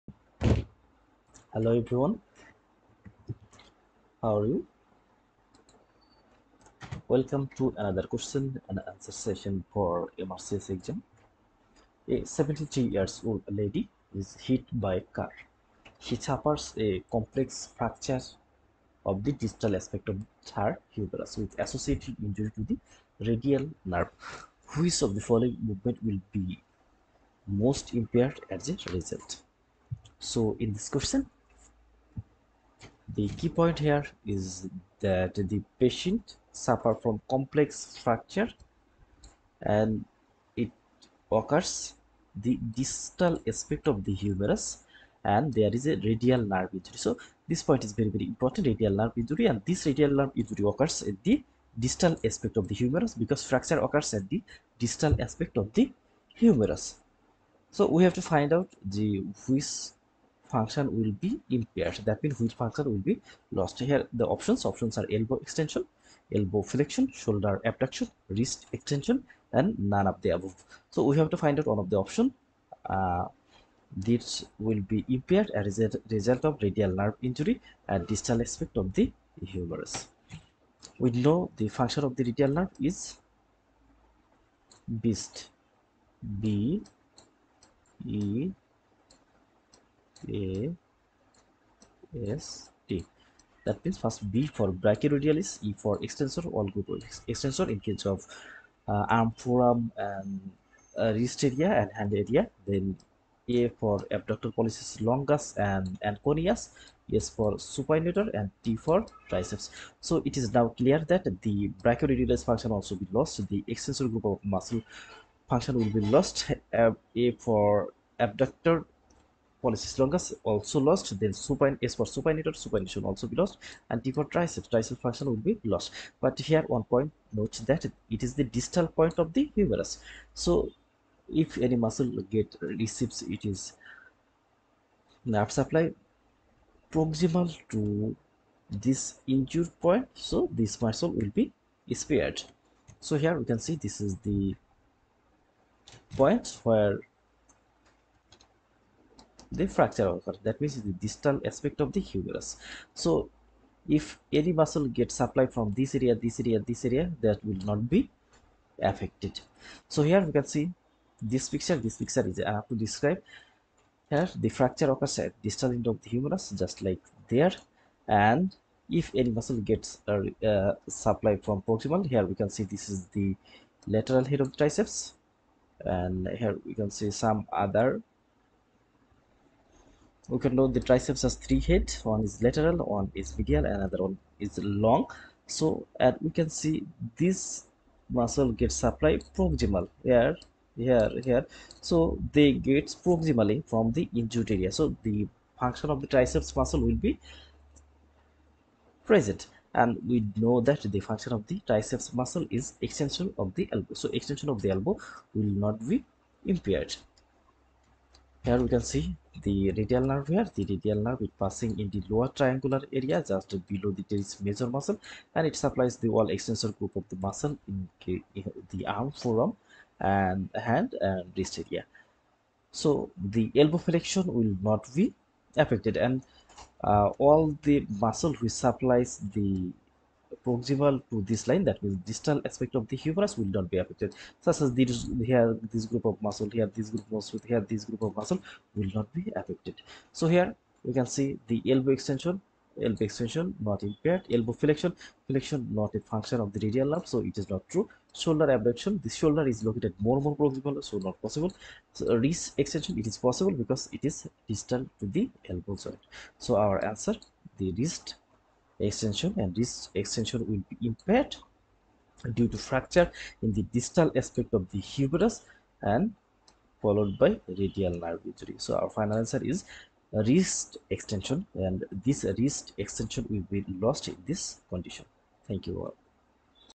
radial nerve injury due to humerus fracture MRCS question solve (CRACK MRCS)
it includes
1. eMRCS videos
2. Fawzia sheet solved
3. Recalls and mores